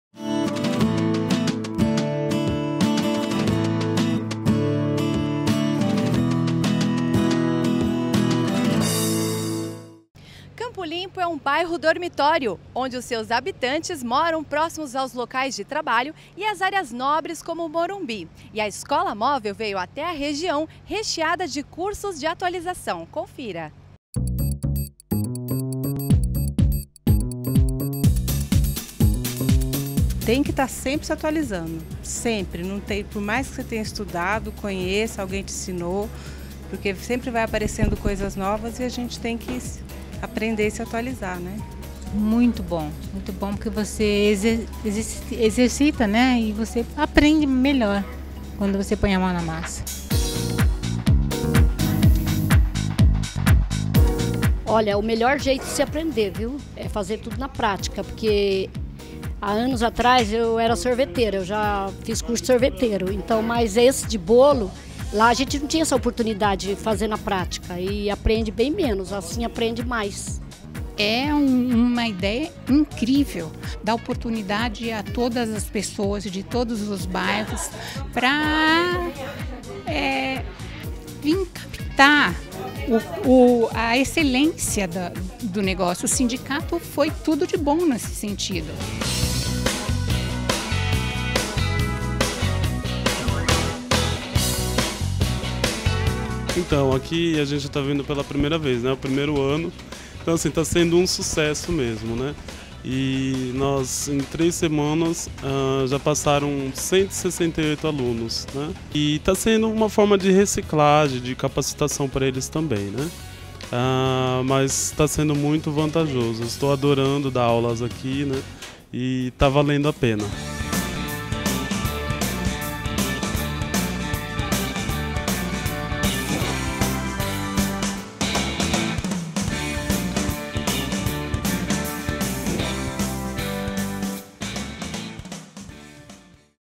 ESCOLA MÓVEL DE PANIFICAÇÃO EM CAMPO LIMPO
A Escola Móvel de Panificação e Confeitaria do Sampapão visitou no mês de agosto de 2015 o bairro Campo Limpo. Quem participou das aulas práticas de bolos como o Naked Cake aprovou a ação. Os cursos são gratuitos para os panificadores e seus funcionários que desejam aperfeiçoar seus conhecimentos e conhecer novas técnicas.